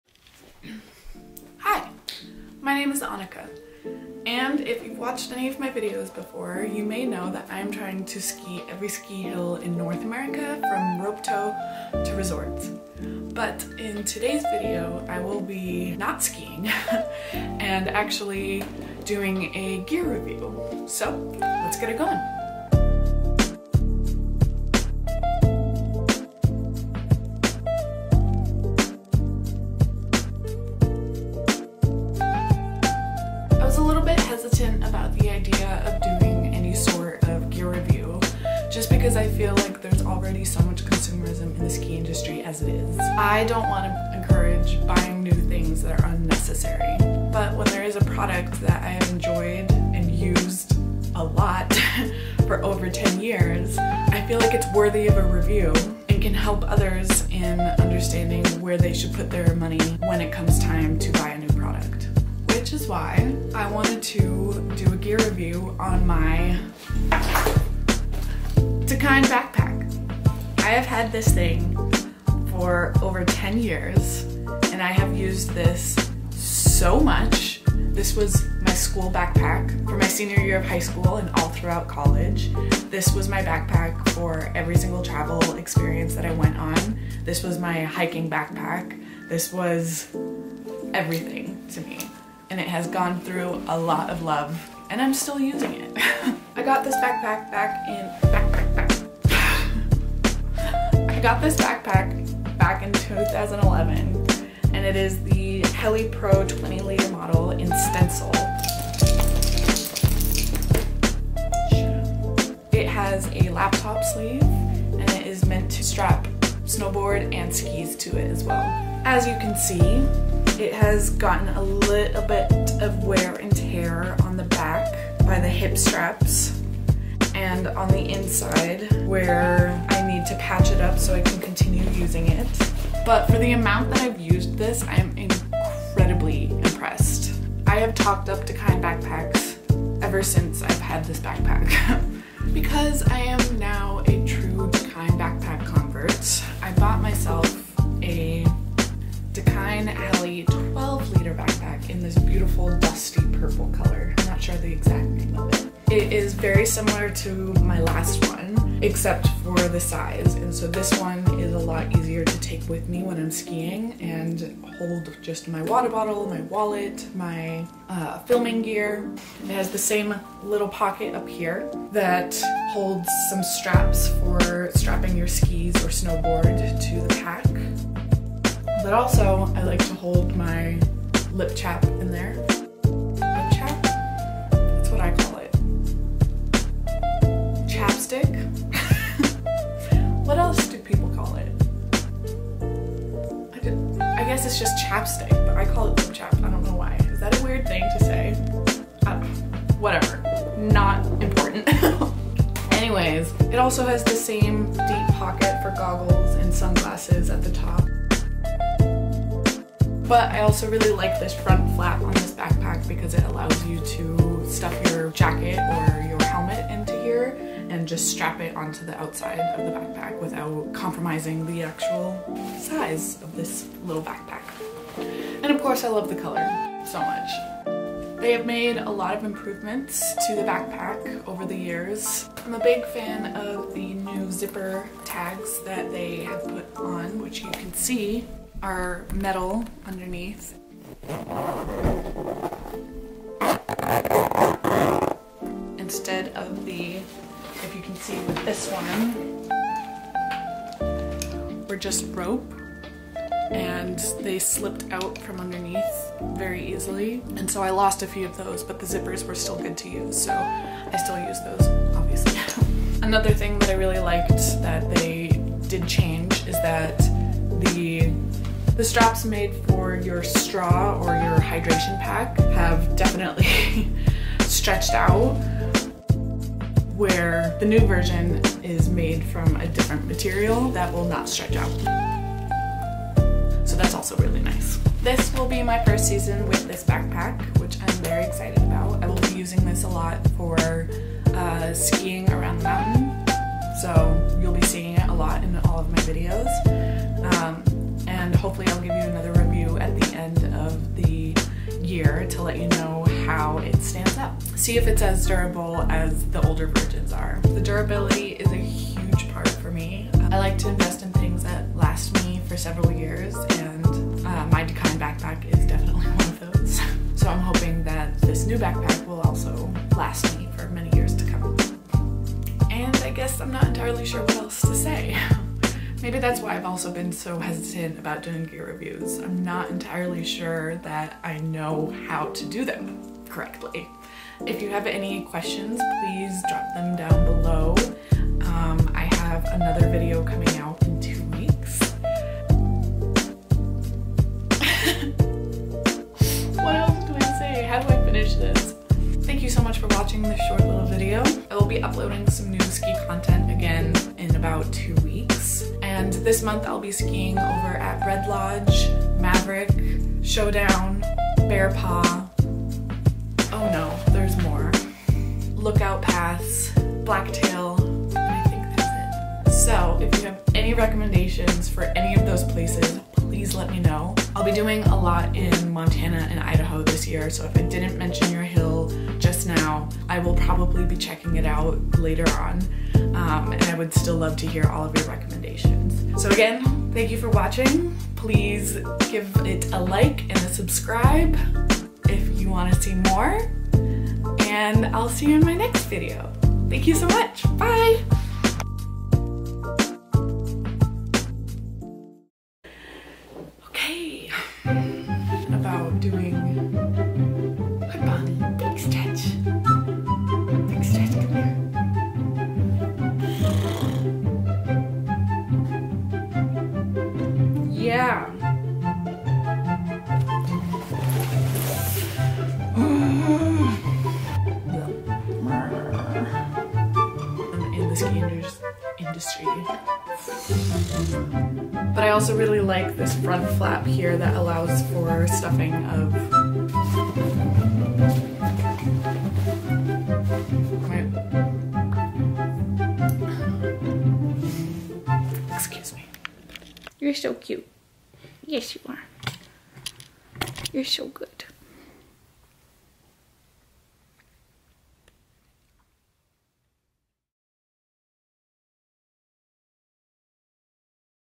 Aanika Skis Dakine Backpack Review
Hello all! Back with another video, but this time, we're trying something a bit different: my first-ever gear review!

I've had my Dakine backpack for over ten adventure-filled years, and it still holds up, so I thought I'd do a little review. I'm new to the review world and I'm not entirely sure if I plan on sticking in it, but I for one rely heavily on YouTube reviews when I'm investing in something new, so I thought I'd put my two cents in here. I'm not about encouraging people to buy new things, but when you are in need of something new, I highly recommend trying to invest in something that will last you for years to come. These bags do that.

Do you have any Dakine products you love or hate? Have a backpack you swear by? Can you count how many times I say backpack?? Let me know below!

Thanks to all who took pictures of me and my trusty backpack!